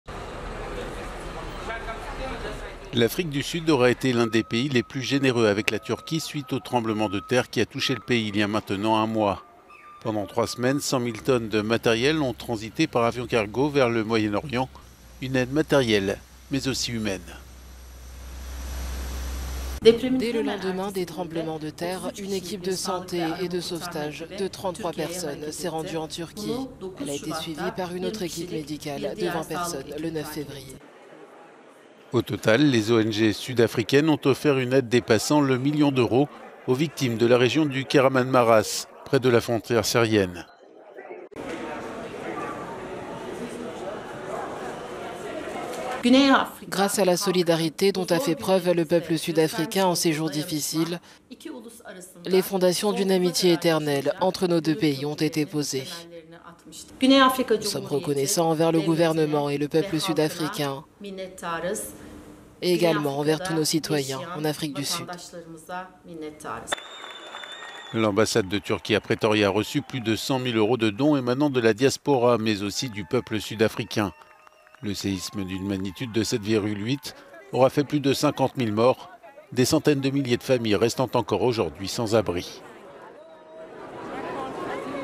La Turquie salue la générosité sud-africaine après les séismes
L'Afrique du Sud aura été l'un des pays les plus généreux avec la Turquie suite au tremblement de terre qui a touché le pays il y a maintenant un mois.